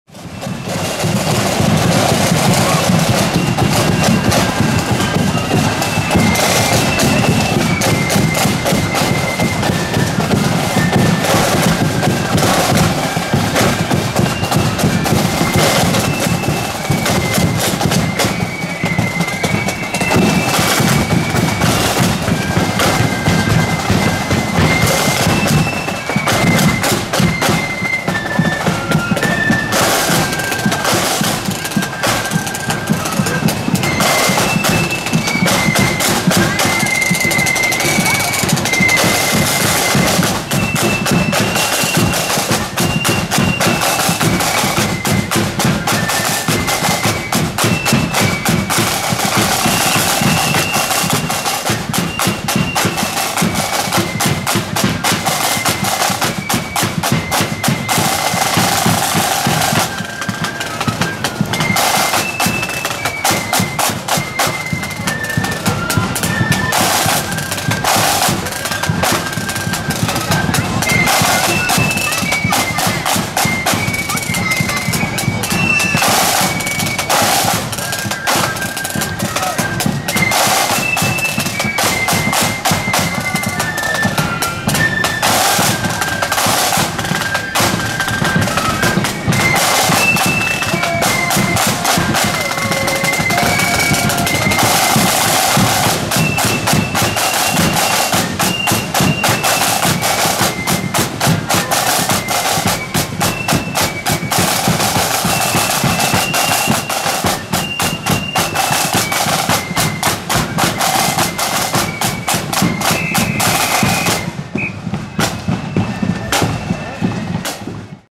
Rising Sons of the Valley @ Armagh True Blues 45th Anniversary Parade 2023
Rising Sons of the Valley On the march in Armagh City for Armagh True Blues 45th Anniversary Band Parade Saturday 19th August 2023